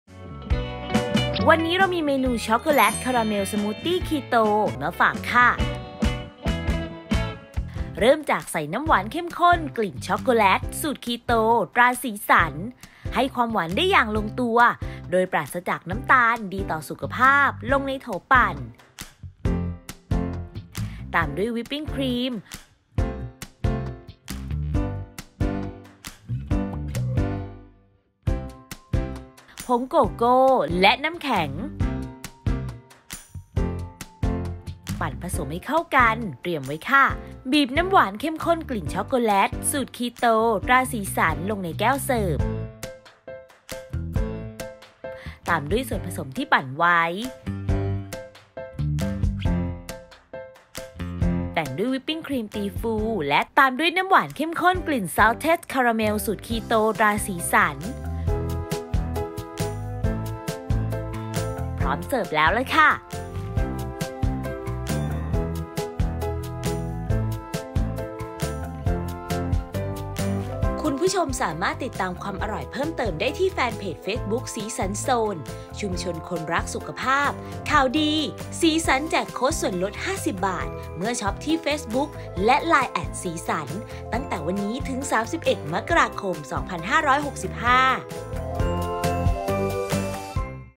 ช็อกโกแลตคาราเมลสมูทตี้คีโต ทำง่ายๆ สไตล์ อ.ยิ่งศักดิ์
ช็อกโกแลตคาราเมลสมูทตี้คีโต (น้ำหวานเข้มข้นกลิ่นช็อกโกแลต สูตรคีโต ตราสีสรร )

น้ำหวานเข้มข้นกลิ่นช็อกโกแลต สูตรคีโต ตราสีสรร (1)    60  มิลลิลิตร
วิปปิ้งครีม   120 มิลลิลิตร
ผงโกโก้    1 ช้อนโต๊ะ
น้ำแข็ง    350 กรัม
น้ำหวานเข้มข้นกลิ่นช็อกโกแลต สูตรคีโต ตราสีสรร(2) วิปปิงครีมตีฟู และน้ำหวานเข้มข้นกลิ่นซอลท์เท็ด คารราเมล สูตรคีโต ตราสีสรร สำหรับตกแต่ง

1.ใส่น้ำหวานเข้มข้นกลิ่นช็อกโกแลต สูตรคีโต ตราสีสรร (1) ลงในโถปั่น ตามด้วยวิปปิ้งครีม ผงโกโก้และน้ำแข็งปั่นผสมให้เข้ากัน
2.บีบน้ำหวานเข้มข้นกลิ่นช็อกโกแลต สูตรคีโต ตราสีสรร (2) ลงในแก้วเสิร์ฟ ตามด้วยส่วนผสมที่ปั่นไว้
3.บีบแต่งด้วยวิปปิ้งครีมตีฟูและน้ำหวานเข้มข้นกลิ่นซอลท์เท็ด คารราเมล สูตรคีโต ตราสีสรร พร้อมเสิร์ฟ

▲Line @yingsakfood